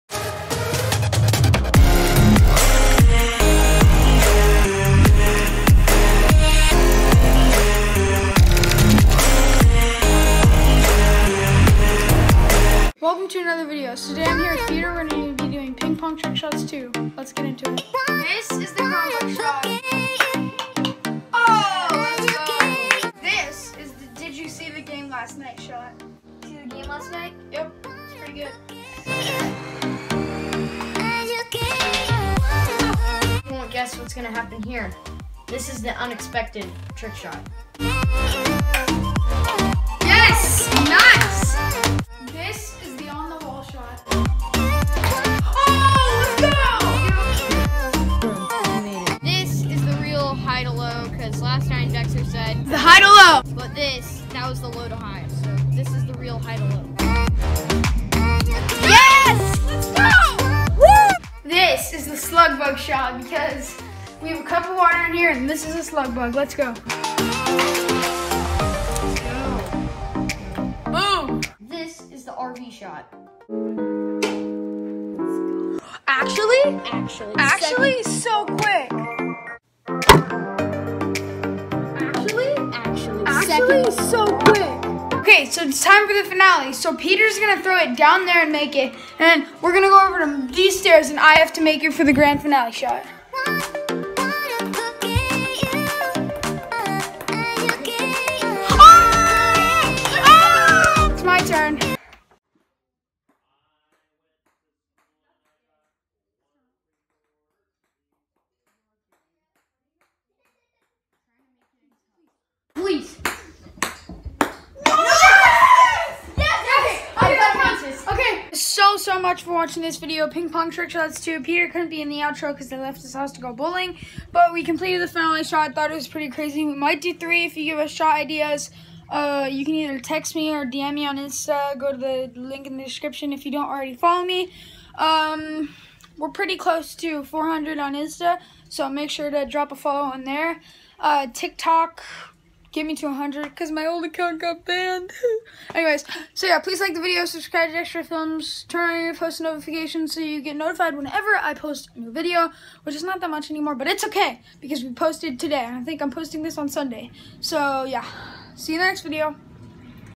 PING PONG TRICK SHOTS 2!!
This was a fun sequel to ping pong trick shots 1, which we filmed last weekend. Comment your favorite shot!